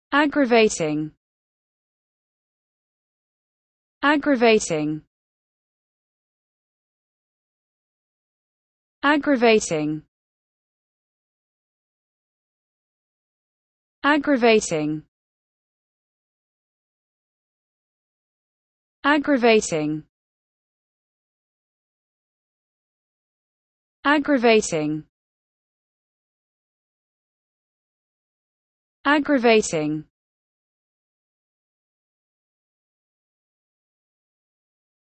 the correct pronunciation of aggravating in English.
This video shows you how to pronunciation aggravating in English.